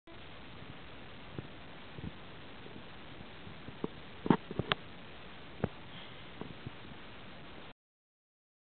My cat Sally drinking in the bath tub at the hotel
When we moved, we had to sleep in 2 hotels rooms before arriving at the new destination. At first we thought she wouldn't go near the bath tub (as she used to drink in it all the time at our last home), but she still wanted to have her water from there. lol